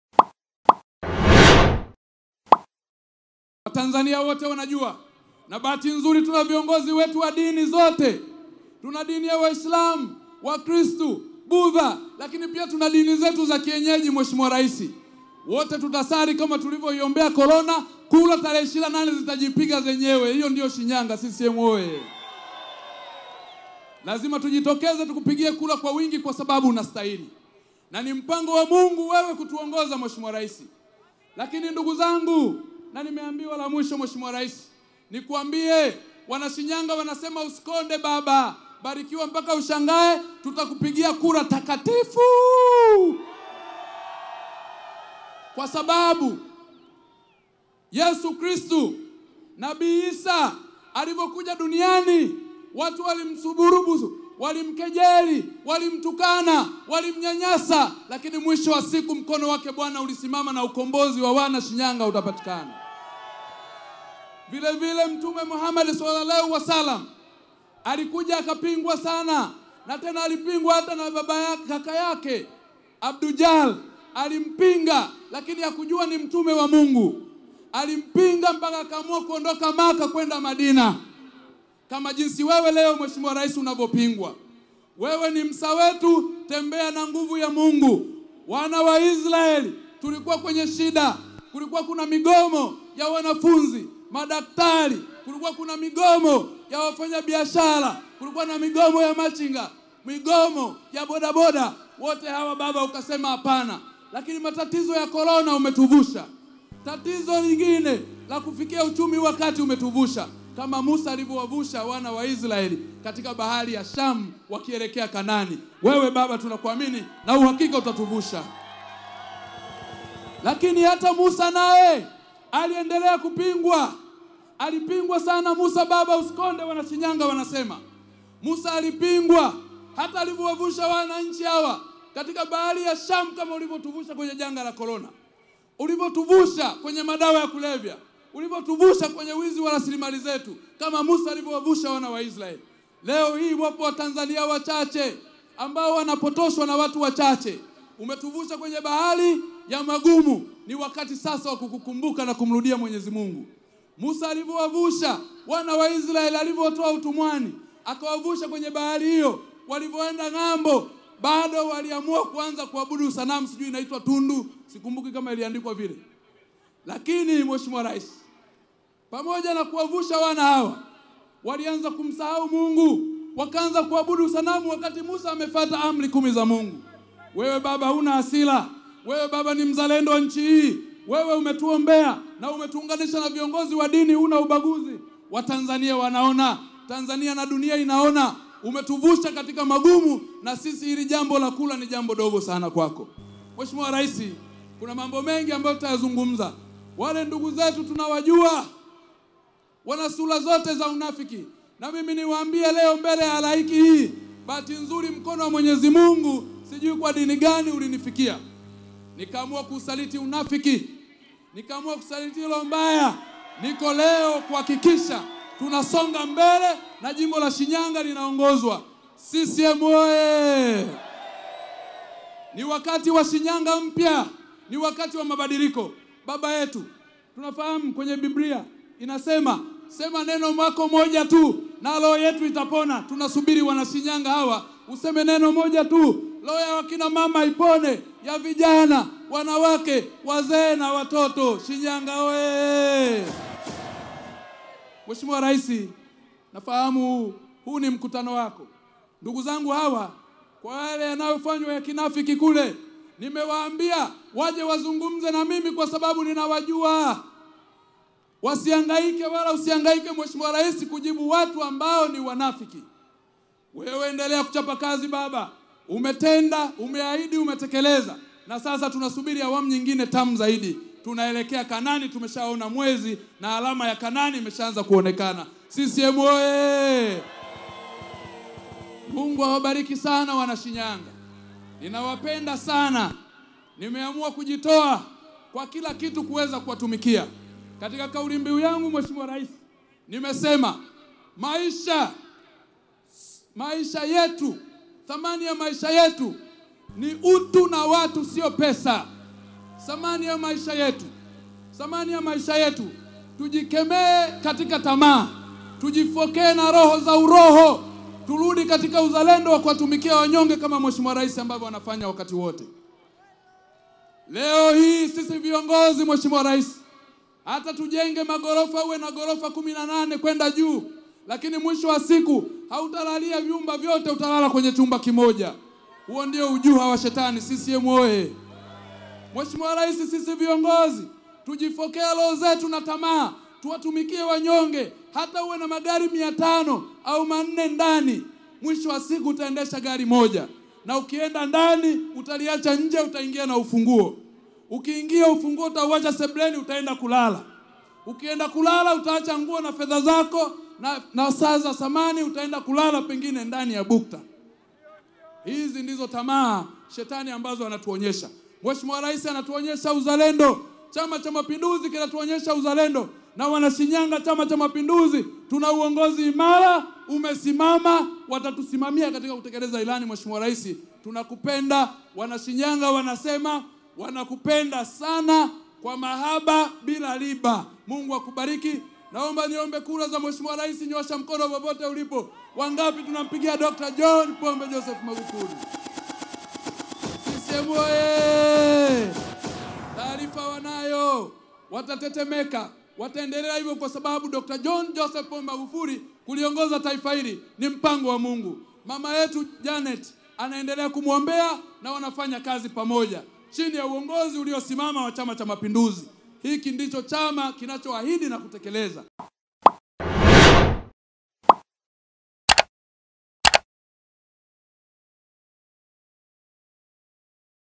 MGOMBEA UBUNGE KATAMBI AFUNGUKA, ASEMA SHINYANGA IMECHELEWA SANA
Mgombea ubunge kupitia tiketi ya Chama Cha Mapinduzi Patrobas Katambi amemueleza Mgombea urais kwa tiketi ya CCM kuwa shinyanga wamejipanga na watahakikisha kuwa anapata kura za kutosha.